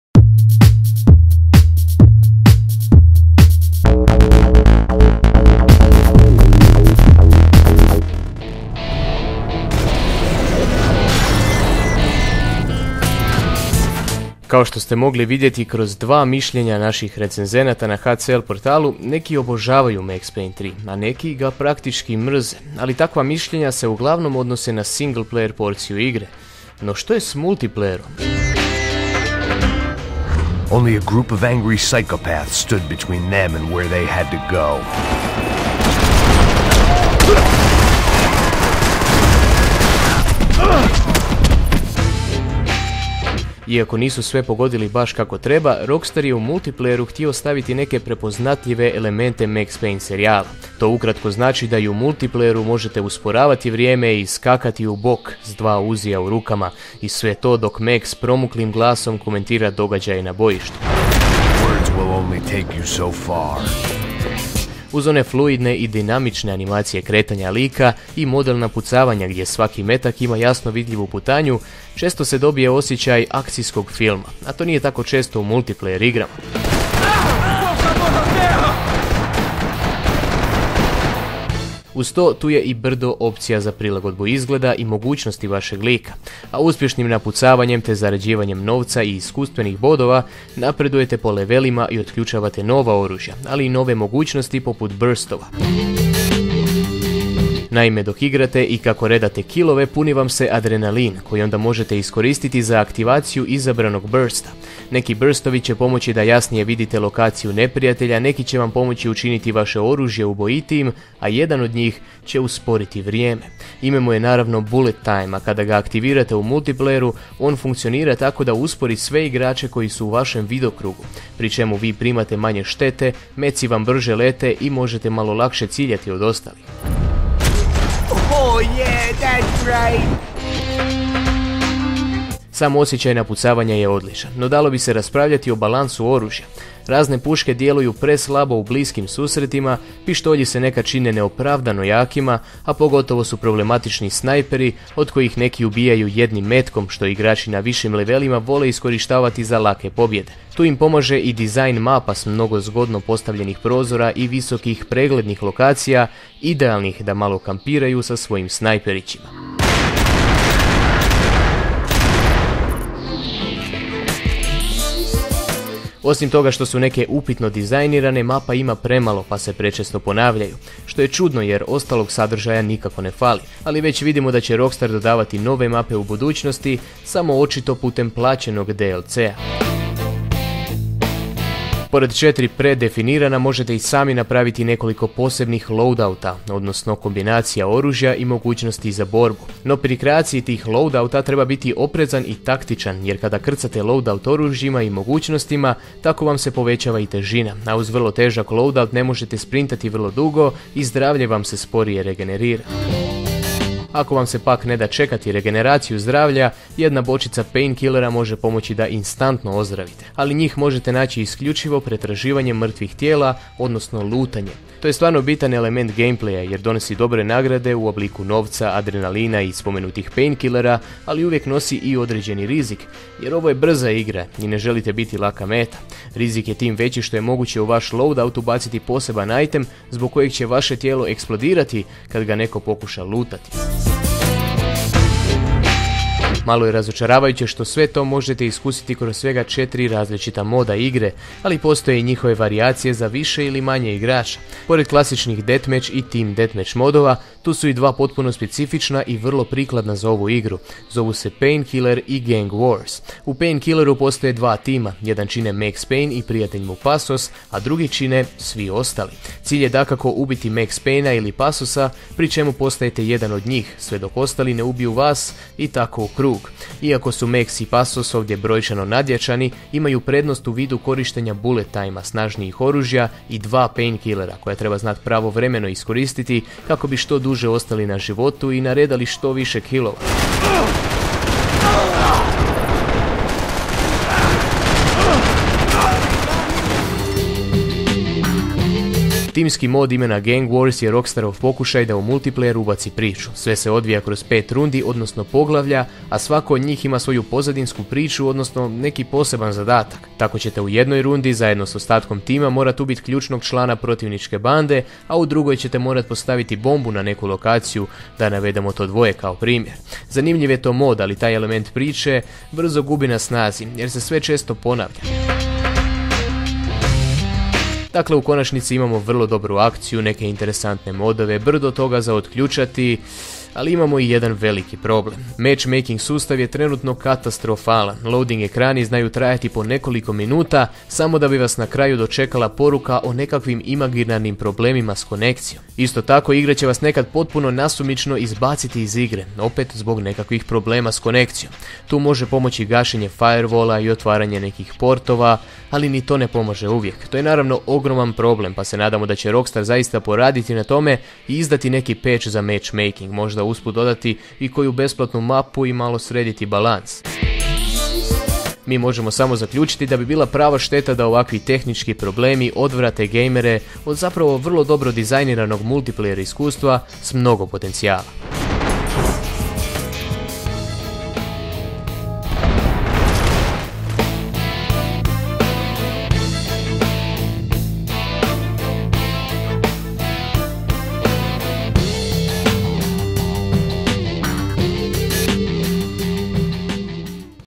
Max Payne 3 multiplayer video recenzija | HCL.hr
Nakon duple i pomalo kontroverzne recenzije singleplayera na HCL-u, donosimo vam i posebnu video recenziju fokusiranu samo na multiplayer Max Paynea 3. Pripremite se za čudne riječi poput burst, loadout i lootanje te nabacite lajk kad završite s gledanjem. U komentarima na HCL-u ili ovdje nam naravno javite svoja iskustva s Max Payne 3 multiplayerom. Mi smo se dobro zabavili, ali mrzimo taj matchmaking!

Osim na YouTubeu i na možete nas pratiti i na raznim društvenim mrežama. Jer smo hip, kul i moderni!